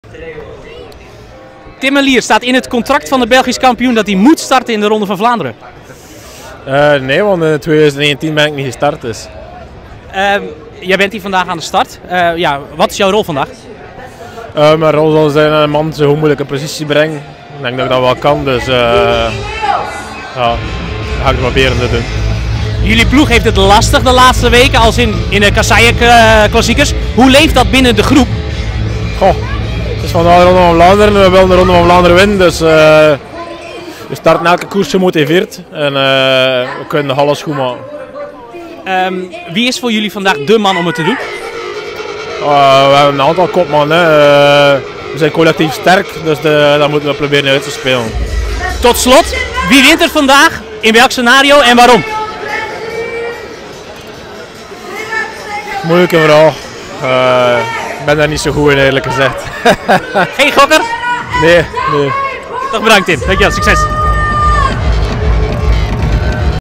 Debutant Tim Merlier: “We willen de Ronde van Vlaanderen winnen”
Tim Merlier maakt zondag zijn debuut in de Ronde van Vlaanderen. De Belgische kampioen wil samen met zijn ploegmaten van Soudal Quick-Step scoren vandaag, want het rest van het voorjaar was nog niet wat ze ervan verwacht hadden. "We willen de Ronde van Vlaanderen winnen."